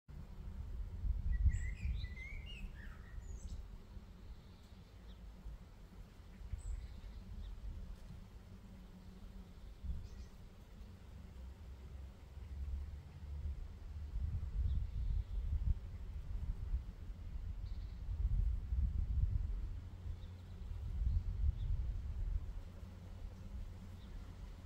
Einen guten Start in die neue Woche !
Kleines Grußvideo mit einem Spielplatz für Schmetterlinge, aus blauer Lavendelblüte, für WhatsApp